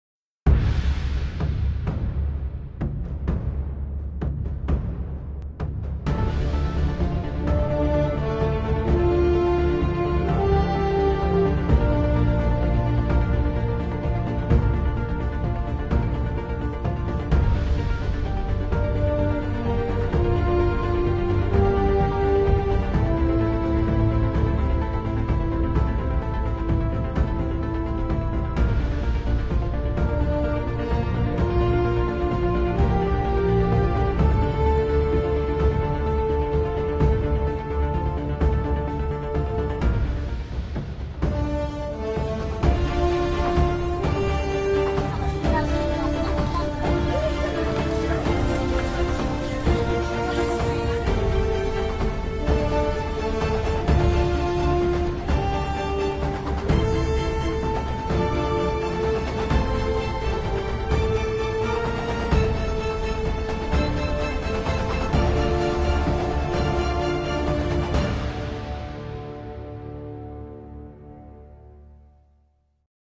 @ CODI And ACRN Resort in tubajon laguindingan
Namasyal Lang sa Resort sa malapit sa amin